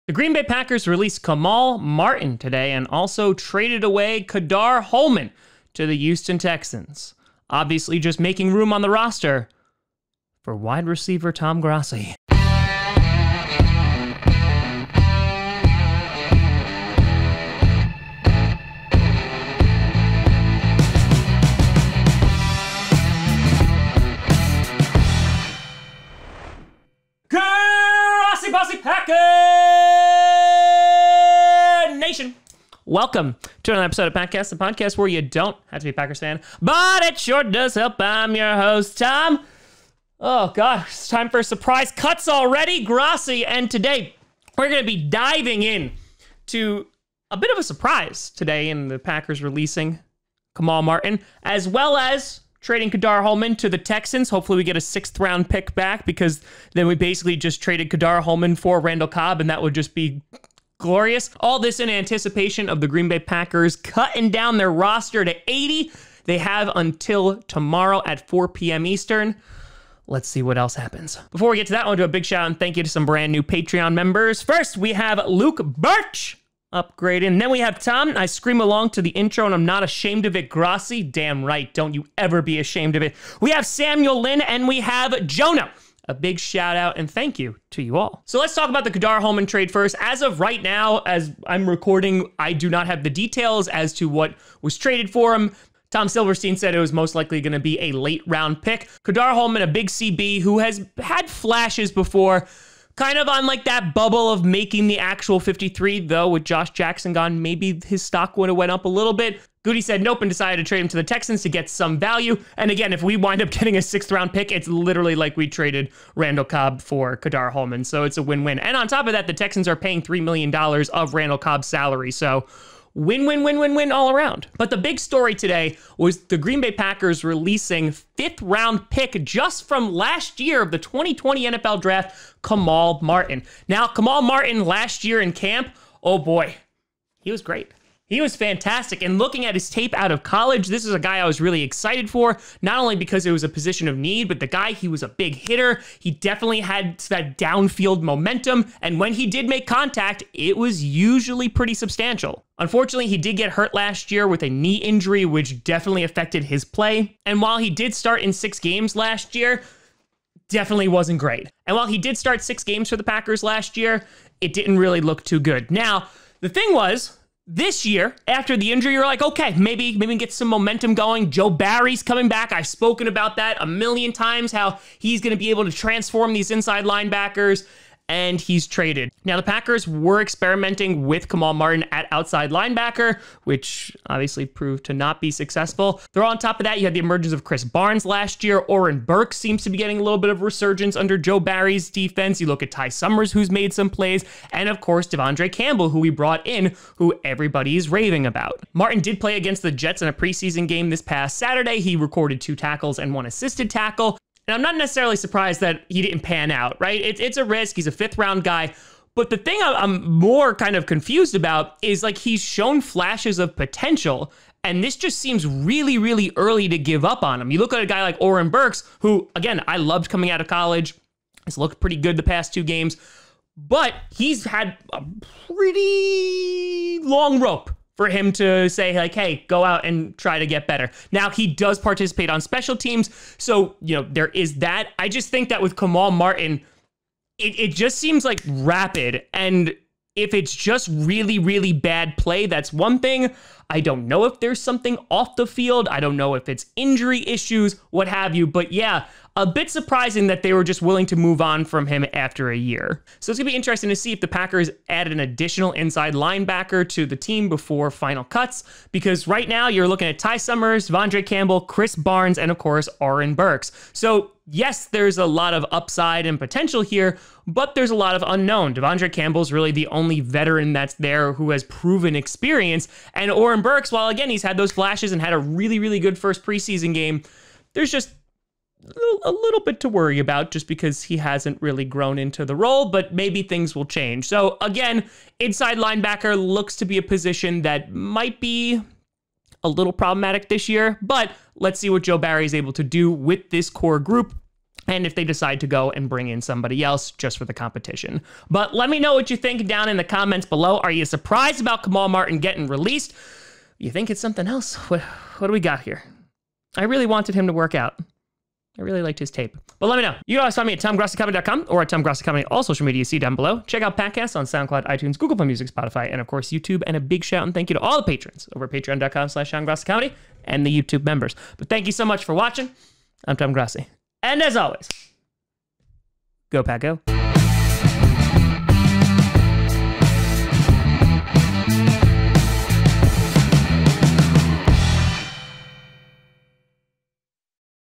Packers Release Kamal Martin & Trade Ka'Dar Hollman
The Green Bay Packers have cut second year ILB Kamal Martin. In addition, they have also traded Ka'Dar Hollman to the Houston Texans.